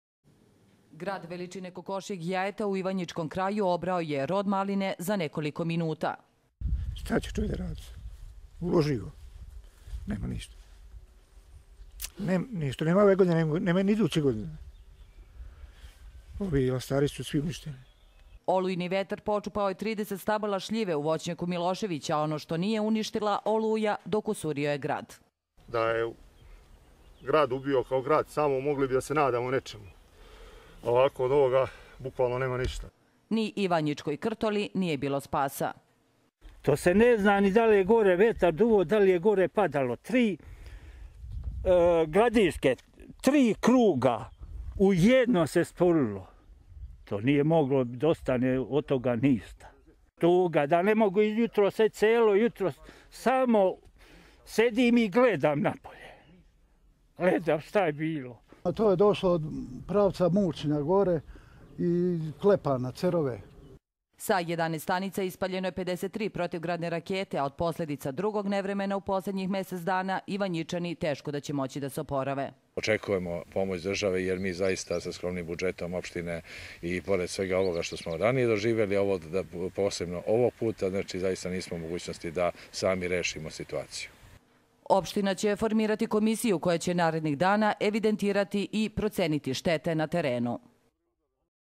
Olujno nevreme praćeno gradom veličine kokošijeg jajeta, opustošilo je ivanjička sela
Iako su protivgradni strelci dejstvovali sa više od 50 raketa, za većinu poljoprivrednika nije bilo spasa. Velika štetu pretrpela je i putna infrastruktura, koja je tek sanirana od posledica katastrofalnih poplava.